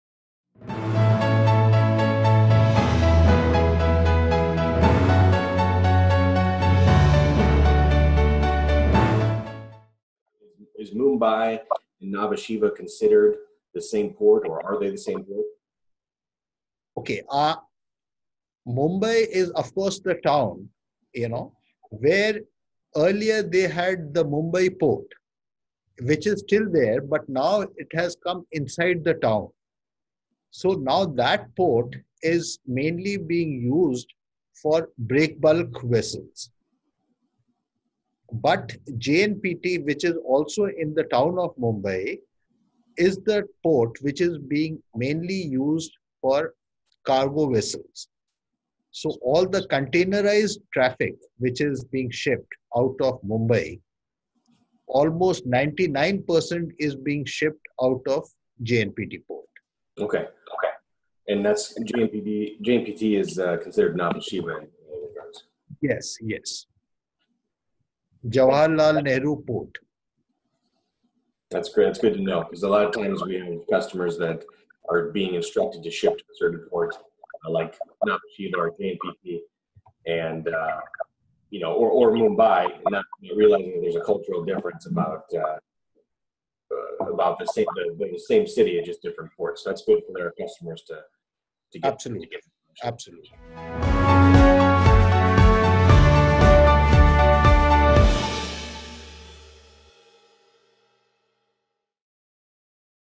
Are Mumbai and Nhava Sheva the same port?
Join us with Scarbrough of USA and Blue Moon Logistics of India as we learn the process of exporting to and importing from India. Learn about: India trade facts, business culture & etiquette, expectations & relationships, regional industry sectors, business opportunities, government parties, regulations, and more! or email